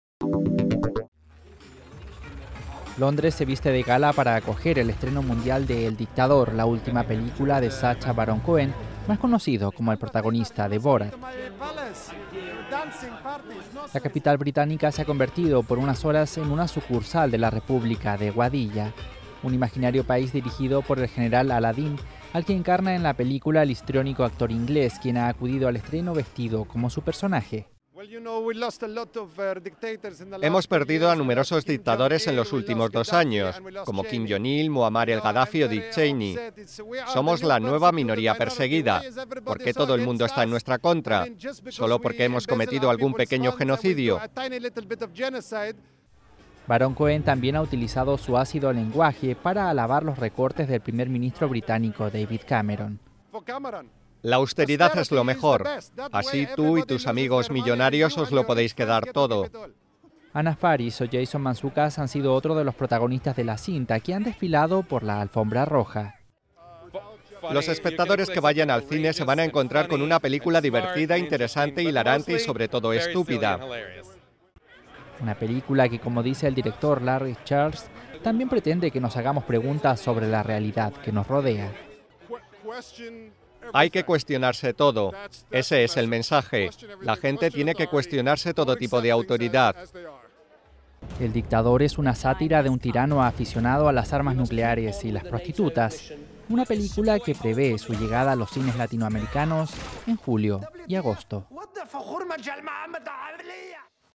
Londres acoge el estreno de "El Dictador"
El actor británico Sacha Baron Cohen desplegó su esperado show en el estreno mundial de la película "The Dictator" (El dictador) en Londres, donde, en su papel ficticio de general Aladeen, no cesó de "amenazar a Occidente". (Video EFE)